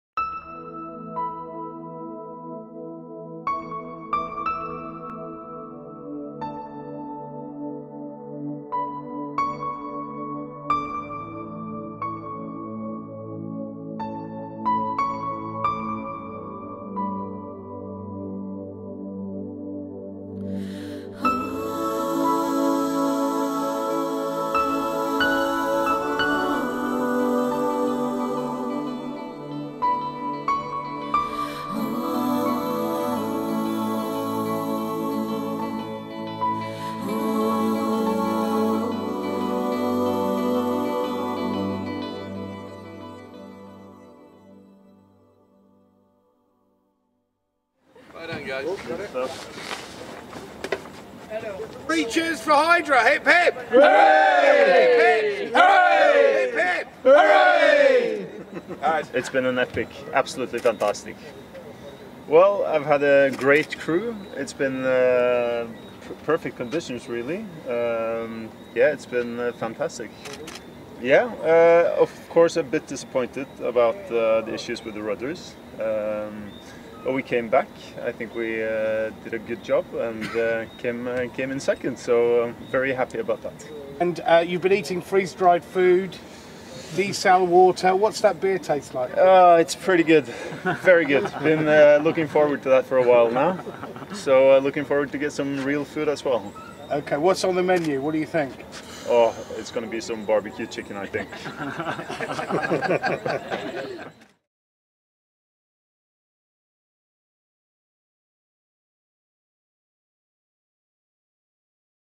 2018 RORC TRANSATLANTIC RACE HYDRA (Henrik Bergesen) ENGLISH
Henrik Bergesen's Norwegian Class40 Hydra, skippered by Tristan Kinloch, finished the RORC Transatlantic Race at 22:02:23 UTC on Sunday, 09 December in an elapsed time of 15 days 10 hrs 2 mins 23 secs. Hydra was runner-up in the Class40 Division to Catherine Pourre's Eärendil. Early in the race, Hydra was very much in contention for the class win until a rudder problem forced the team to make a pit stop in the Cape Verde Islands. After completing the race, Hydra's team received a warm welcome on the dock from RORC CEO Eddie Warden Owen and Race Officer Steve Cole. Marina Manager Charlotte Bonin greeted the team with ice cold beers and presented the crew with a basket of Grenadian goods, including rum and chocolates, courtesy of the Grenada Tourism Authority.

It's been an epic race; absolutely fantastic," commented Henrik Bergesen dockside at Camper & Nicholsons Port Louis Marina. "I have had a great crew and we had perfect conditions. We had an issue with the rudders, but we came back and did a good job; we kept going and got back into second place, so I am very happy about that. After eating freeze dried food and desalinated water, the beer tastes really good. We are looking forward to some real food; barbecue chicken I think! After this race, we're well prepared for the RORC Caribbean 600 in February and it should also be great fun."